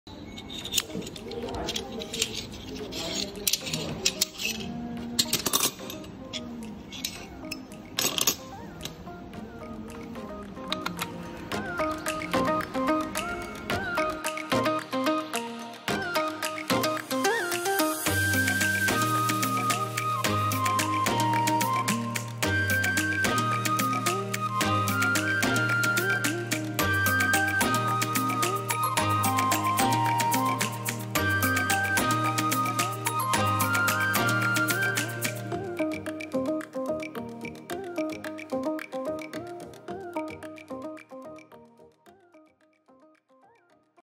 iPhone 11 Won't Turn On Repair
The pseudo-soldering of the middle layer has caused the boot-up problem. Check out this clip to see how we fix it.